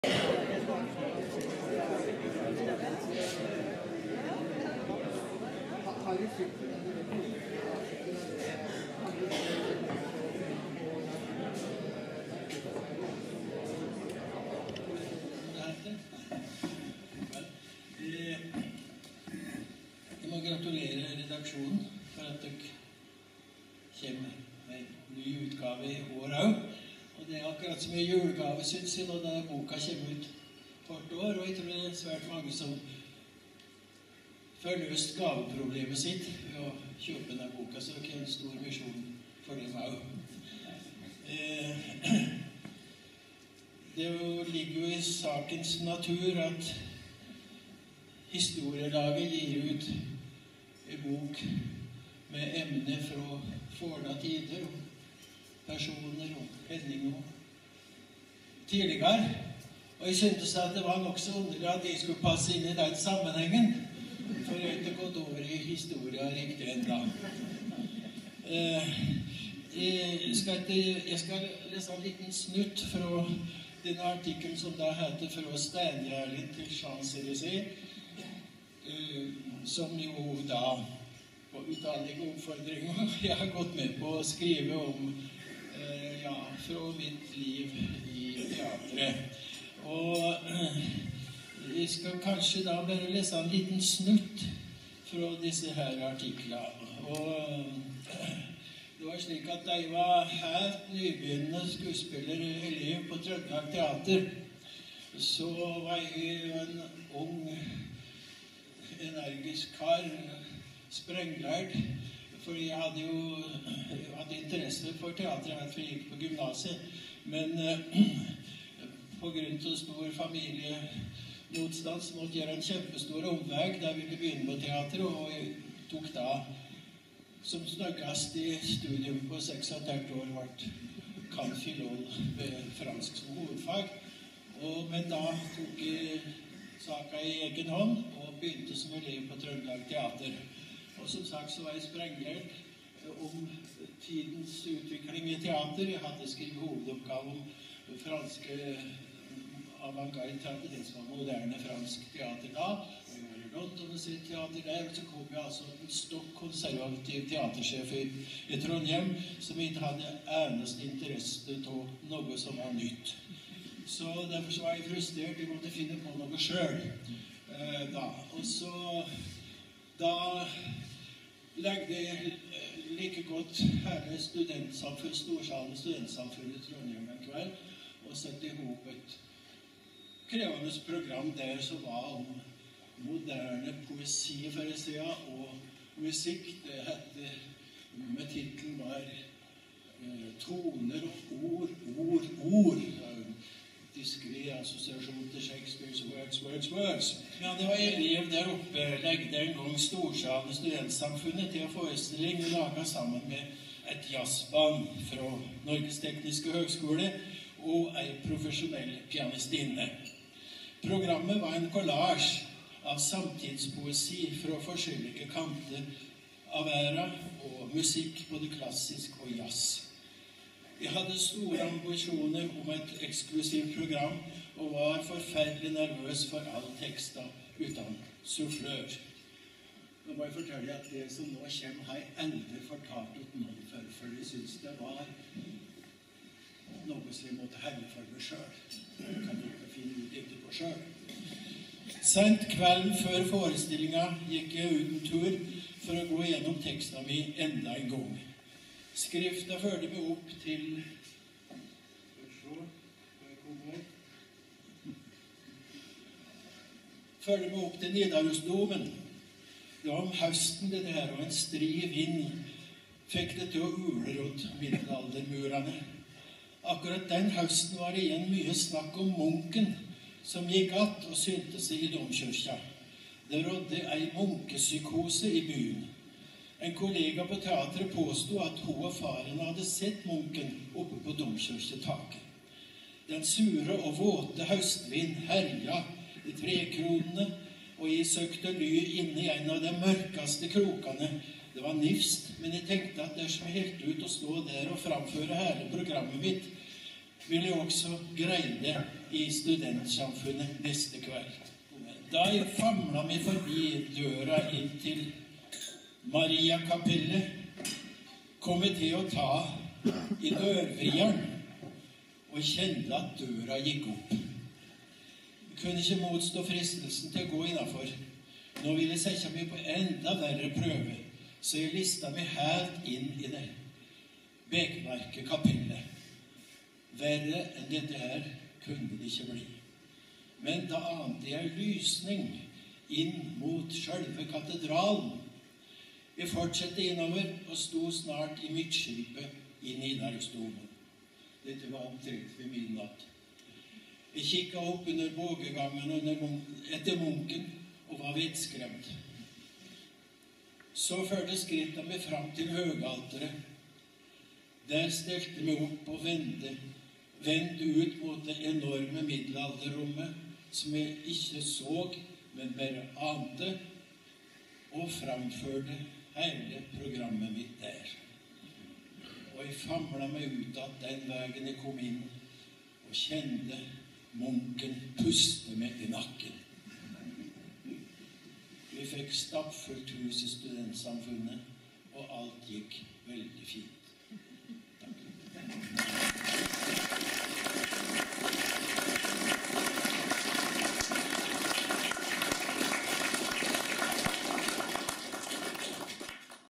Per Tofte les frå artikkelen i Fronsbygdin 2015
Per Tofte les frå artikkelen sin i Fronsbygdin 2015